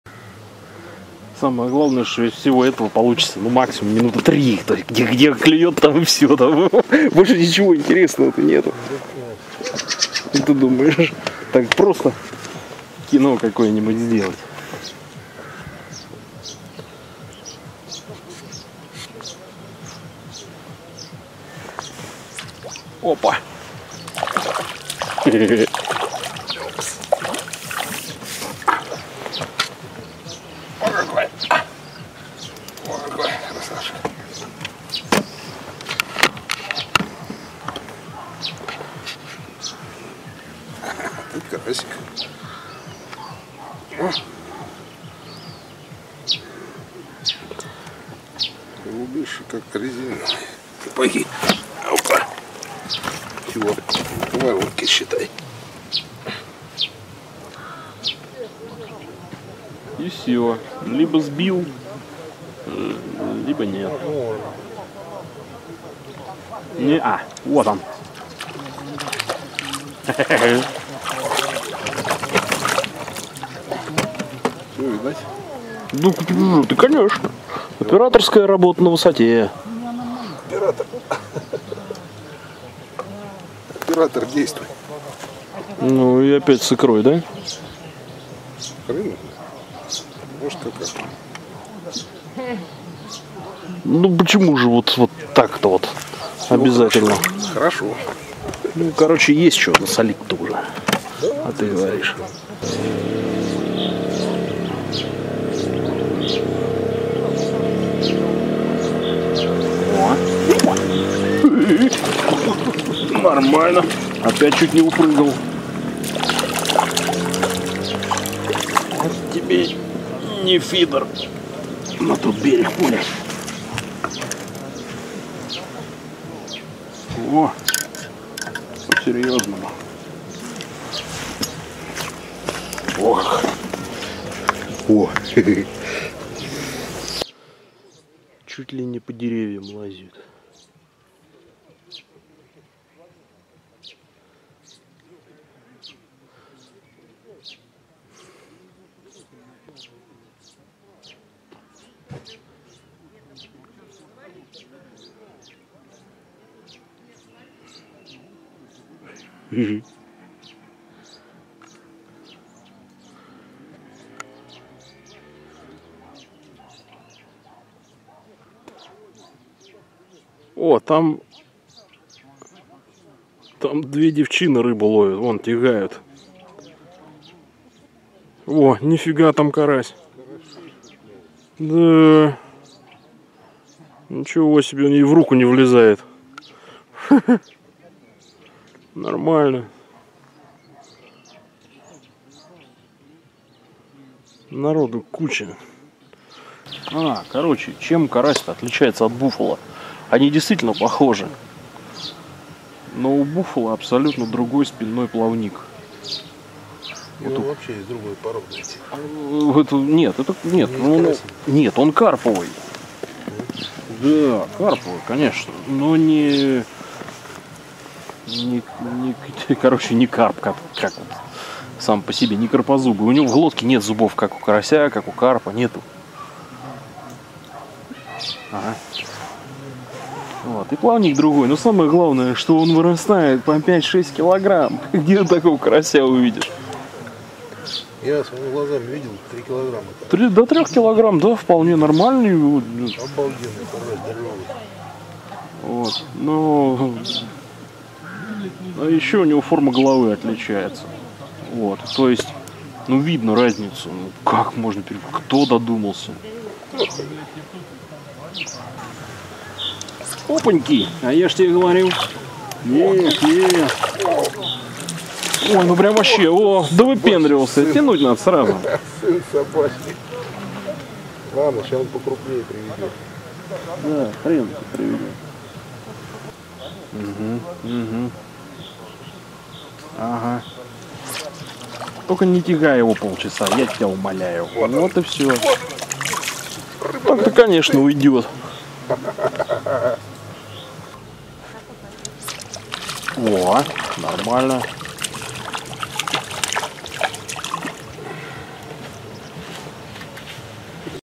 Первая вылазка в этом году
Ловим карасика в Тульской области. Насадка - хлебушек и мастырка. Червь и тем более опарыш давно оставлены всяким любителям по полчаса спать. Народу на водоёмах очень много,прямо прорвало! Правда, клюёт не у всех, что особенно доставляет на фоне постоянных плюхов батиных уловов =)